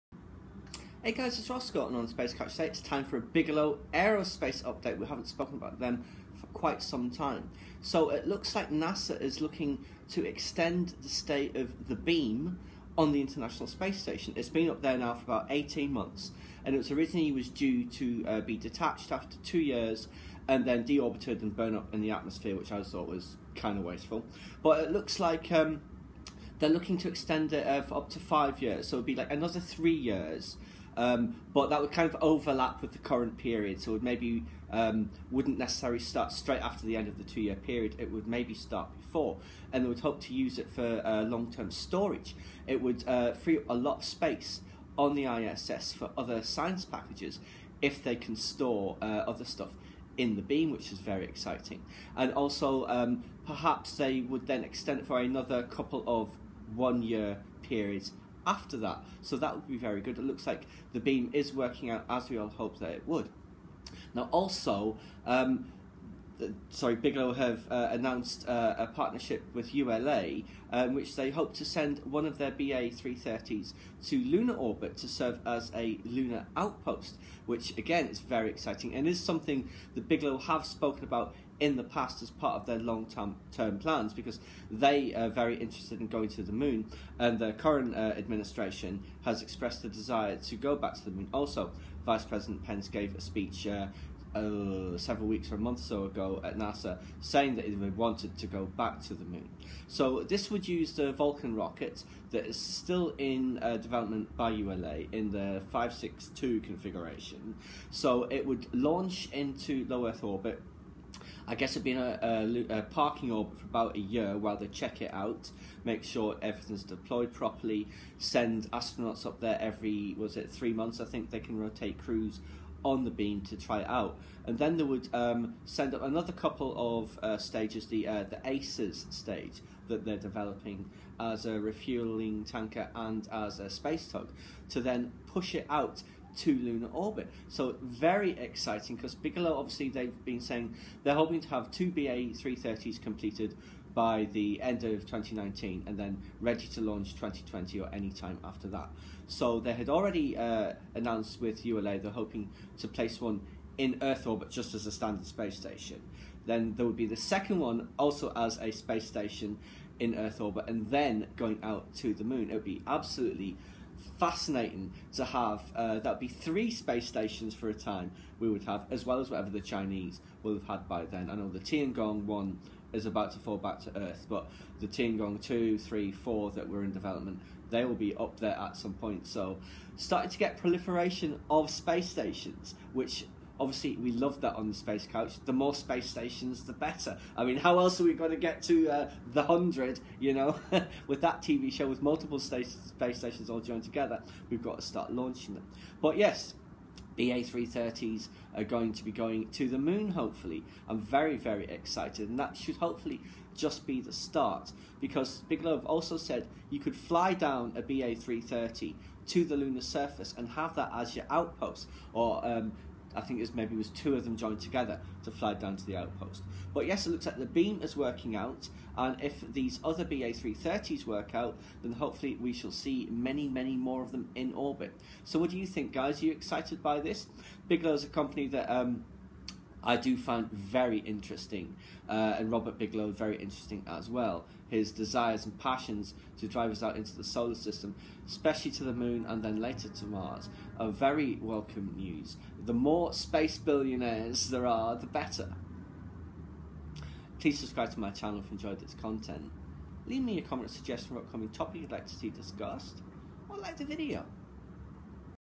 Bigelow Aerospace update October 2017
Bigelow are planning to partner with ULA to deploy one of their BA330s to lunar orbit to serve as a depot and gateway to the Moon in 2022!